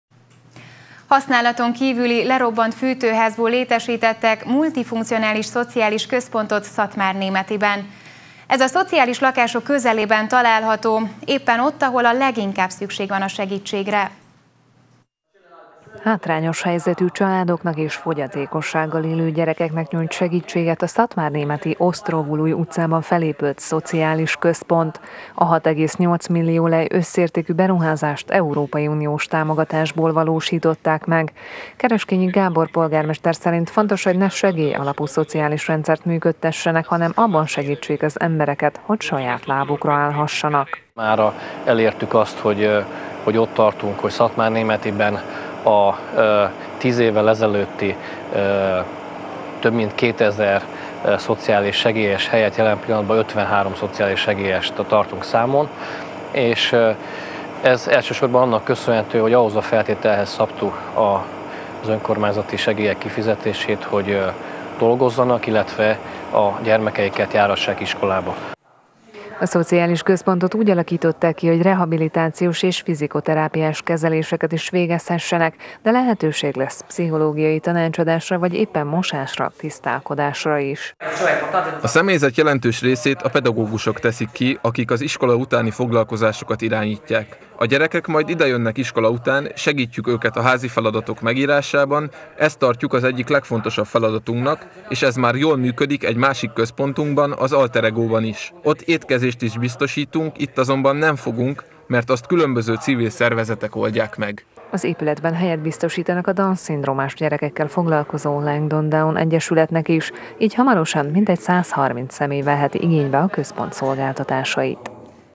Kazánházból szociális központ – Erdélyi Magyar Televízió
Használaton kívüli, lerobbant fűtőházból létesítettek modern, multifunkcionális szociális központot Szatmárnémetiben. Ez a szociális lakások közelében található, éppen ott, ahol a leginkább szükség van a segítségre.